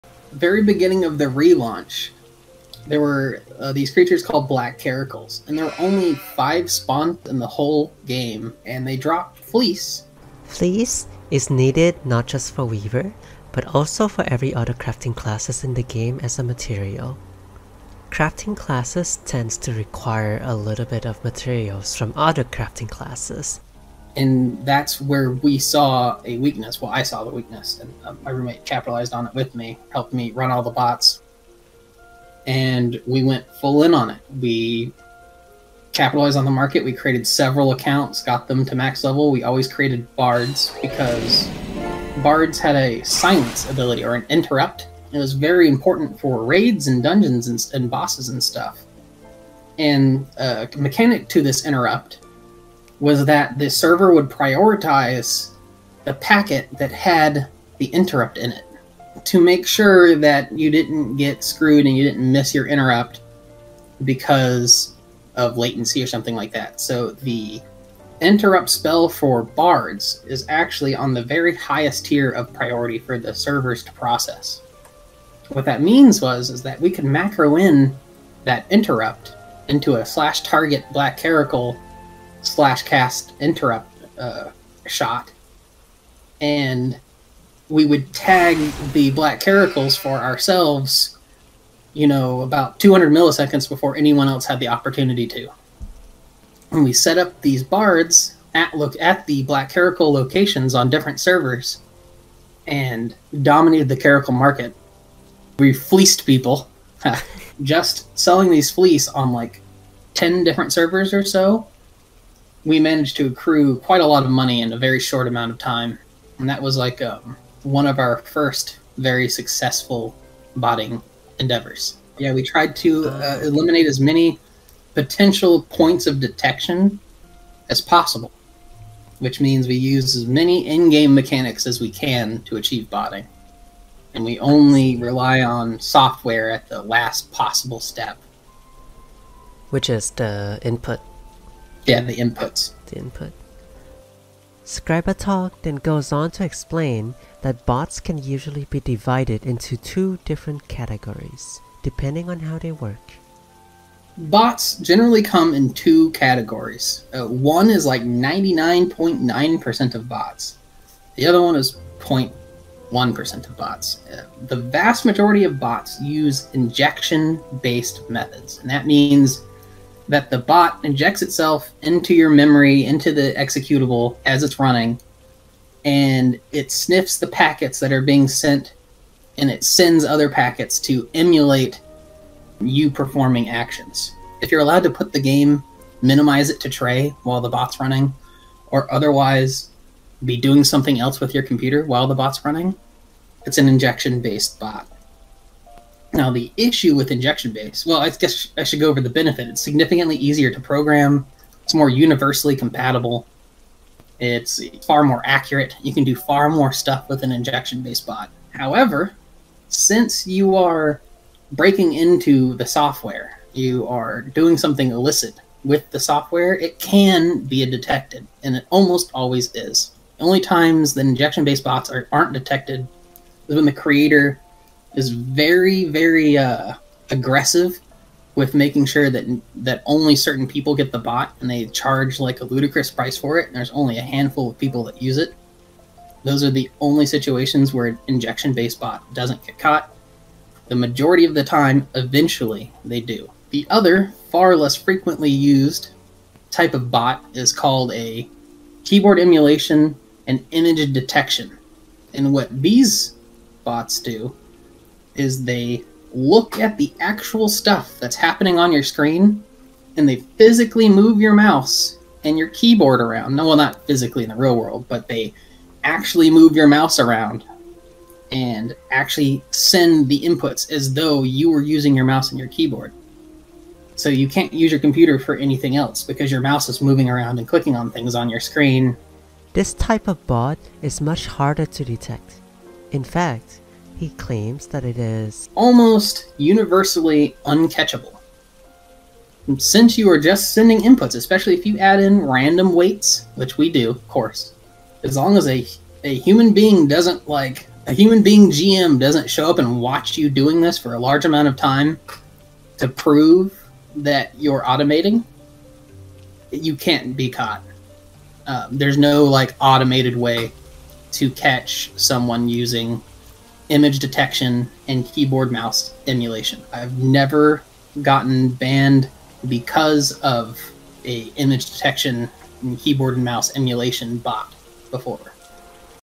[Clip] [Tells from PF] Wool's Errand
Botting is a frequently discussed topic in Final Fantasy XIV. But who writes them? Who uses them? Today, we talk to someone who write bots for a living.

Please give me your thoughts and comments on the show, I'd love to hear from you guys.

Special thanks to Scrybatog for coming to the show and talking about his experiences.

A transcription of the episode can be found here.